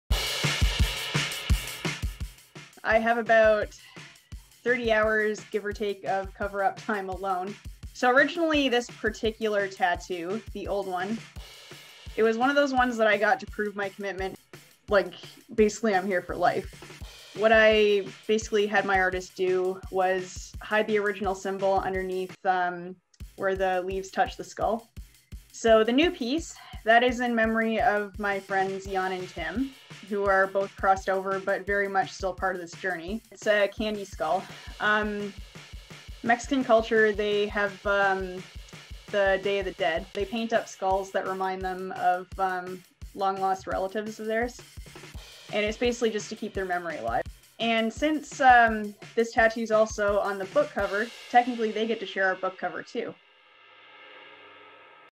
Walking Away from Hate Cover Story
Lauren Manning, as a troubled teenager, covered her body with white power tattoos to show her commitment to the white supremacy movement. Ultimately, she managed to walk away from hate, and has transformed her life and her tattoos. Read her story, and that of her mother who never gave up on her daughter, in Walking Away from Hate: Our Journey through Extremism, published May 2021 by Tidewater Press.